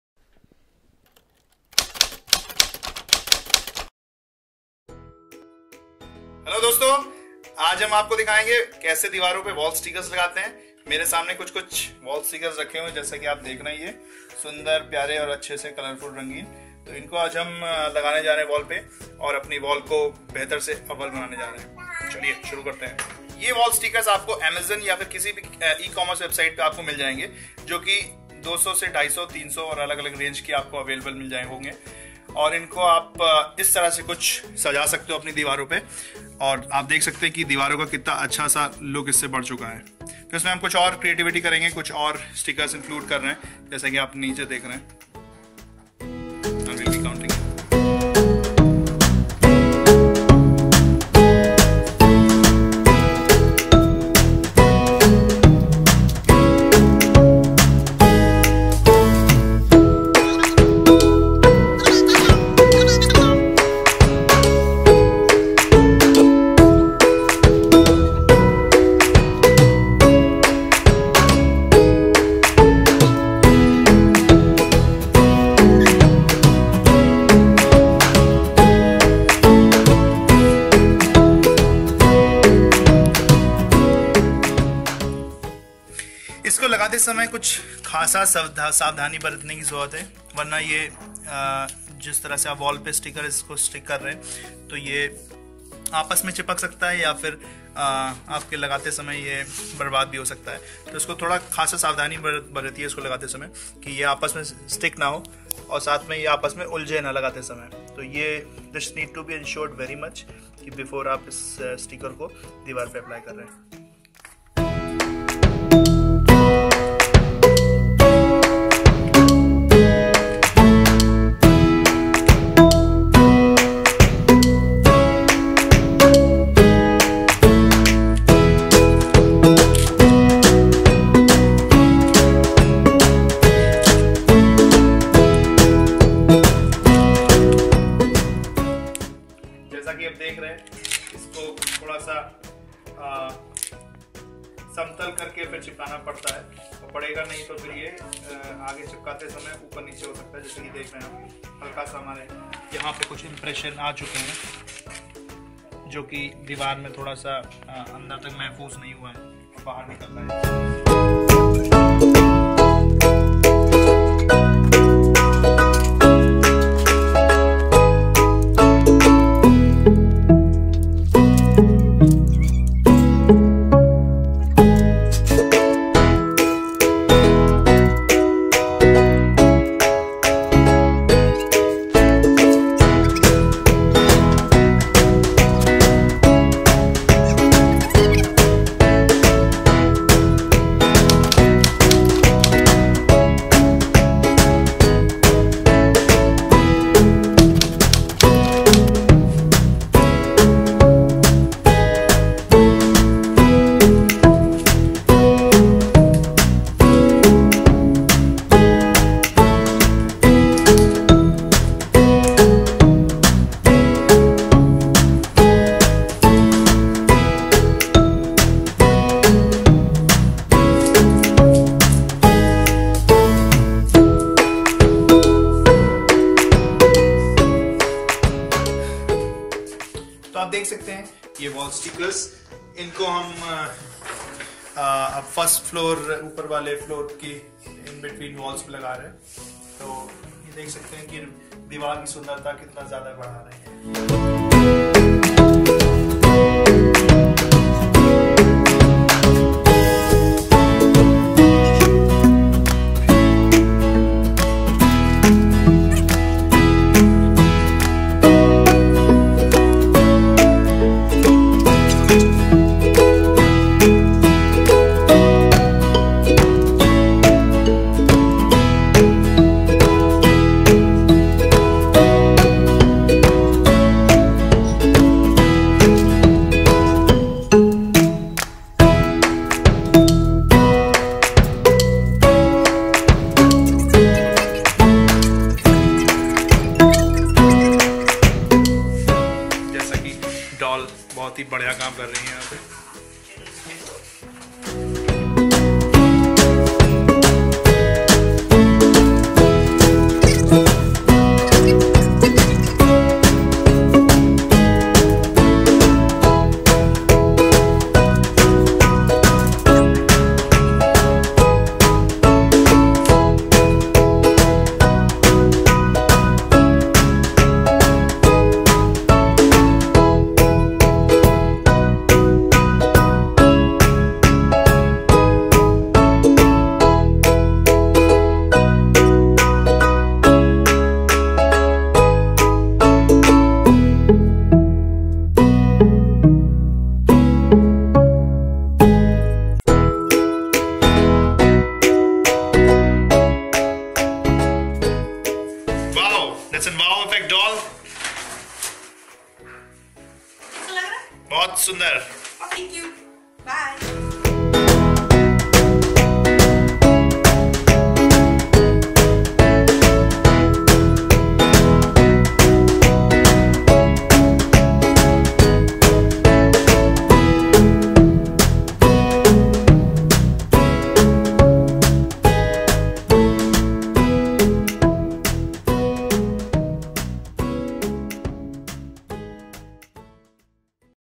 Wall Stickers - Flamingo Bamboo, Honeybee Butterfly on Flowers, Bird Cage
A wall decal, also known as a wall sticker, wall tattoo, or wall vinyl, is a vinyl sticker that is affixed to a wall or other smooth surface for decoration and informational purposes. Wall decals are cut with vinyl cutting machines. Most decals use only one color, but some may have various images printed upon them.

Wall Stickers are the nice way to decorate the walls and to put it on wall is itself a complicated and careful task, lets see how we approach this fabulous